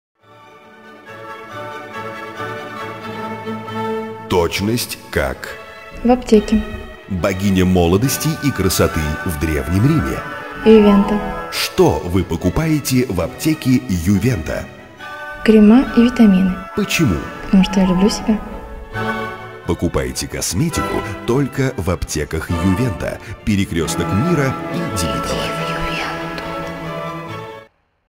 Market-Studio.com Аптека Ювента ( Zenova ) 28 sec
Рекламный ролик Аптека Ювента  ( Zenova ) 28 sec, 2006 г.

Advertising Agency: Market-Studio.com
Creative Director: Любовь Бабуева